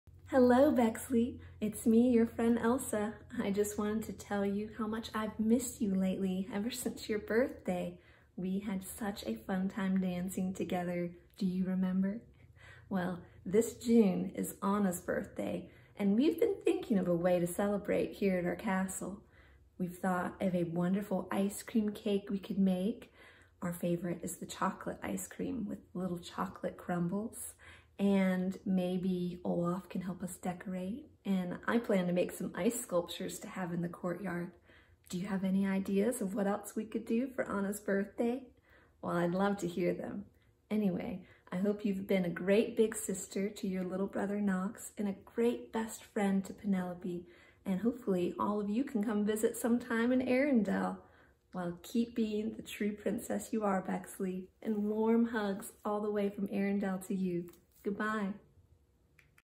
Bexleigh’s Special Message
The Ice Queen has a special message for Princess Bexleigh!

To book your video greeting, visit LittleGlassSlipperWV.com.